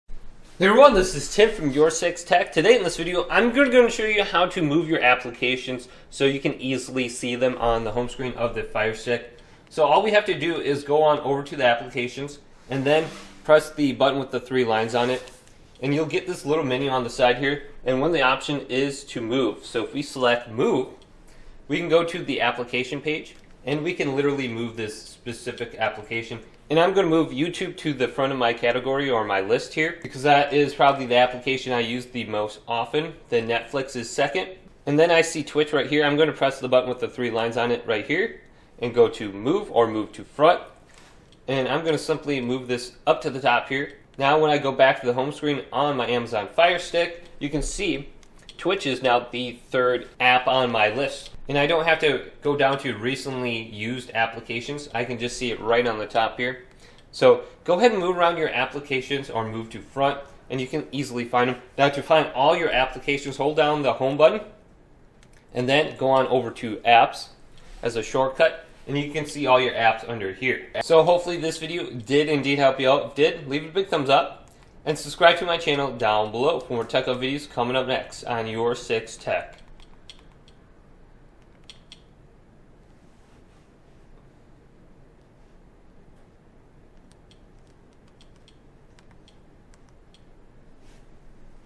How to Move Apps on Home screen on Amazon Fire TV Stick (Easy Method)
Do you want to know how to move apps on the home screen of the amazon firestick so you can see your most used apps or recently used apps! To do this press the button with the 3 lines on it over the app and the there will be an option to move the app! You rearange the order of the list and move the app to the front of the list!